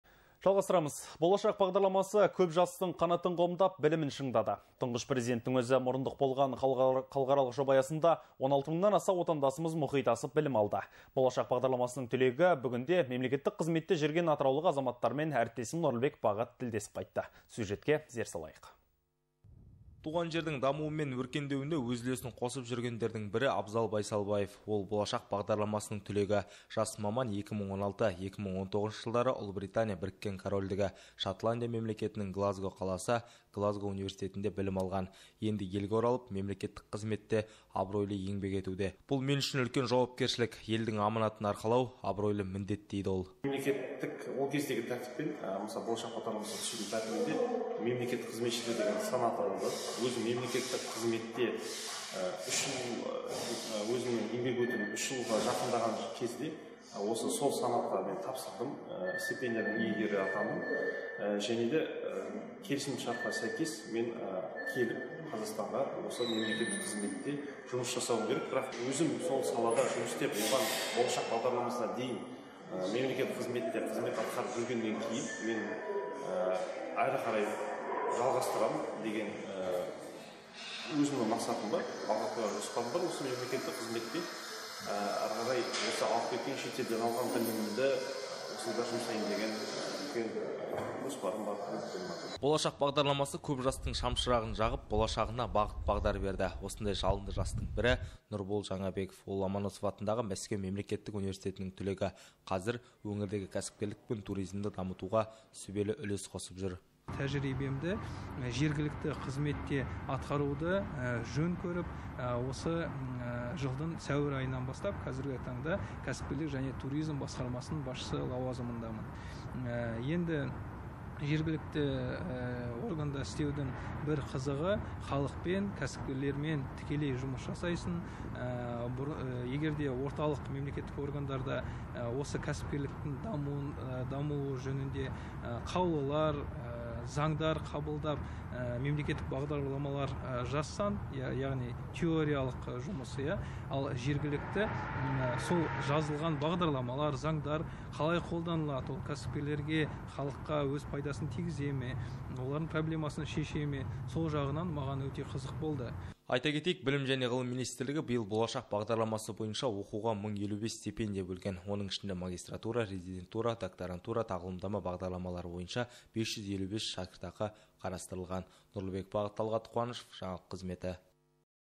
Ел үмітін арқалаған жастар
Болашақ бағдарламасы - көп жастың қанатын қомдап, білімін шыңдады. Тұңғыш Президенттің өзі мұрындық болған халықаралық жоба аясында 16 000-нан аса отандасымыз мұхит асып білім алДЫ. Болашақ бағдарламасының түлегі, бүгінде мемлекетік қызметте жүрген Атыраулық азаматтармен әріптесім Нұрлыбек Бағыт тілдесіп қайтты. Сюжетке зер салайық.